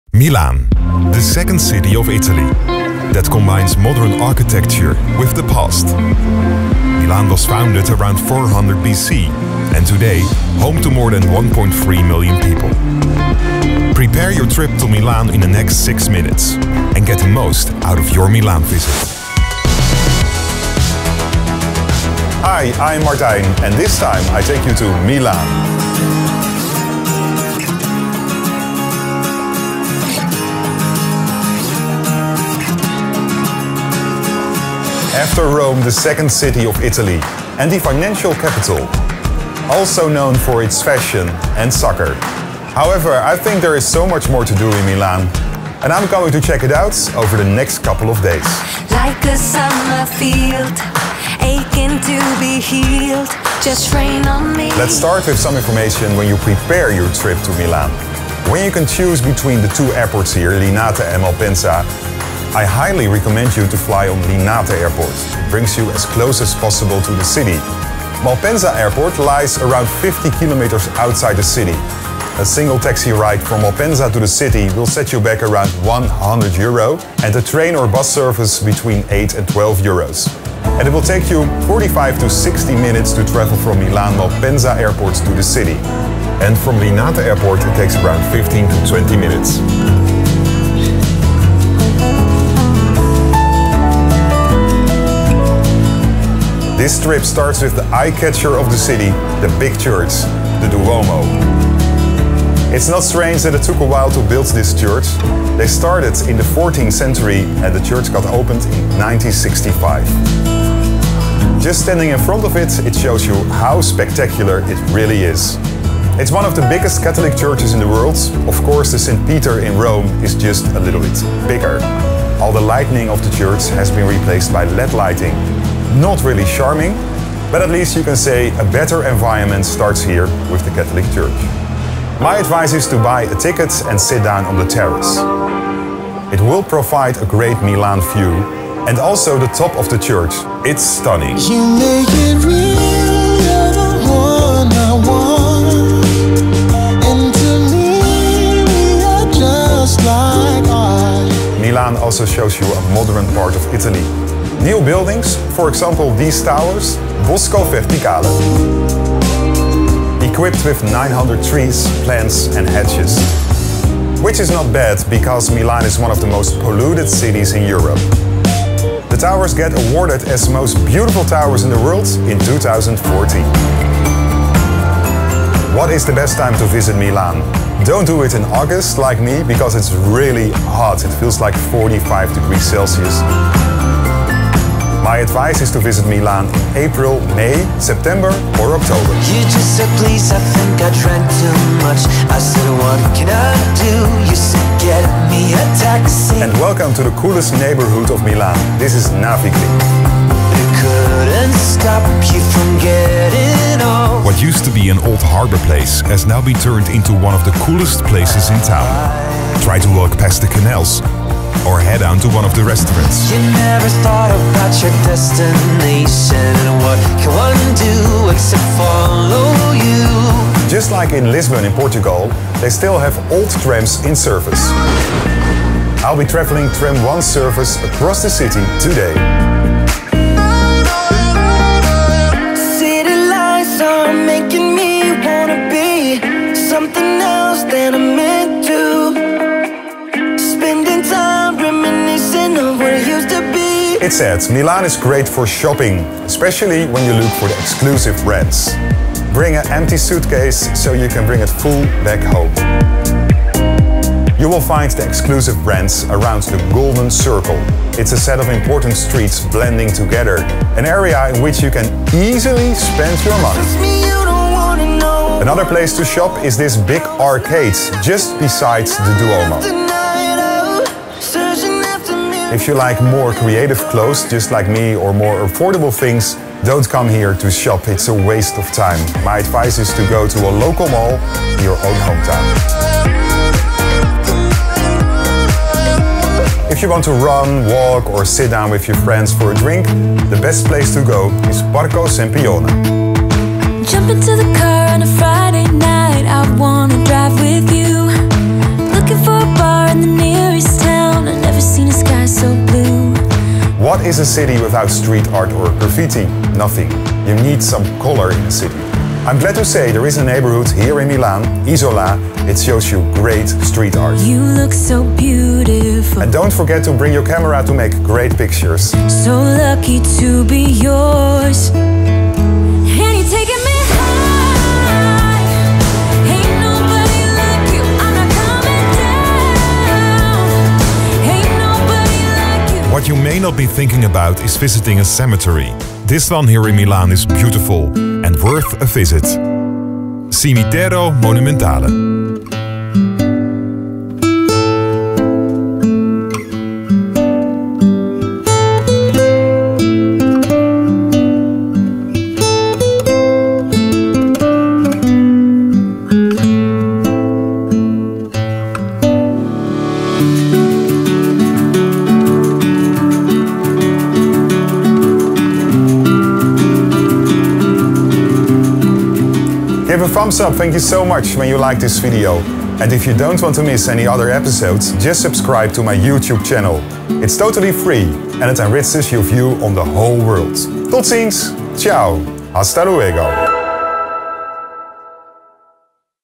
Milan Travel Guide - TRAVEL GUIDE MILAN IN 6 MINUTES
Milan Travel Guide in only 6 minutes! Milan is recognized as one of the world’s most important fashion capitals, but it also has a wealth of interesting museums and things to see and do. The Milan Travel Guide shows you in only 6 minutes all the attractions of this beautiful Italian city.

▬ What to do in Milan,  Travel Guide Milan  ▬▬▬▬▬▬▬▬▬▬
0:00​​​ - Intro
0:55​​ - Arriving information (airport)
1:35​ - Duomo di Milano
2:31 - Bosco Verticale
2:55​​ - Best time to visit (weather)
3:15​​ - Navigly
3:39 - Tram service
4:03​​ - Shopping
4:46 - Parco Sempione
5:03 - Isola (Street art)
5:32 - Cimitero Monumentale

Gear:
Panasonic GH5
Lumix 42.5mm f 1.7
Lumix 8-18mm f 2.8-4.0
Lumix 15mm f 1.7
Lumix 35-100mm f2.8
iPhone X Film mic pro
DJI Ronin S
DJI Osmo